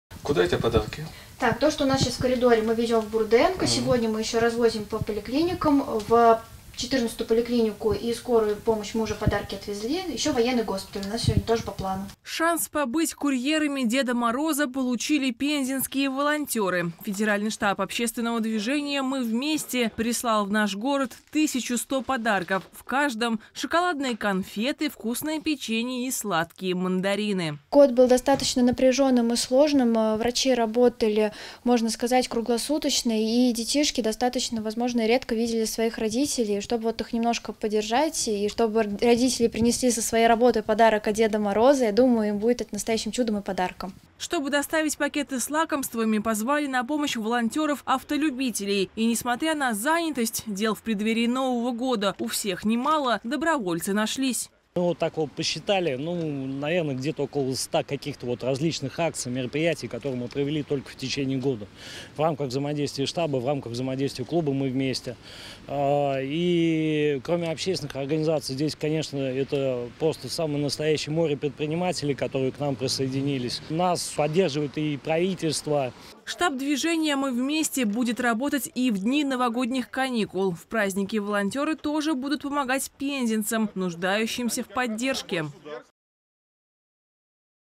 Дети пензенских врачей получат сладкие подарки на Новый год
Для детей пензенских медиков, которые в 2020 году, в силу сложившихся обстоятельств, видели своих родителей не часто, приготовили подарки. Только в областную больницу имени Бурденко доставили более пятисот пакетов со сладостями. Привезут их и в другие лечебные учреждения региона.